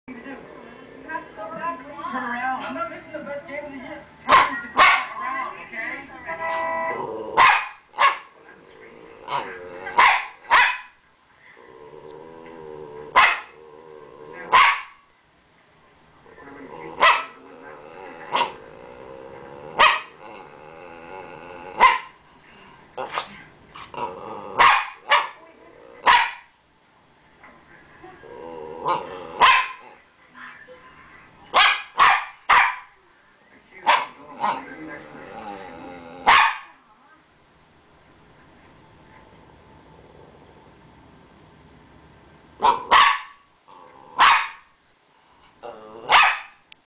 This is my Rat Terrier, Champ :)
I just found this on my computer & decided to upload it.

Believe it or not, this is his playful attention-seeking bark, as you can see by the wagging of his stub of a tail.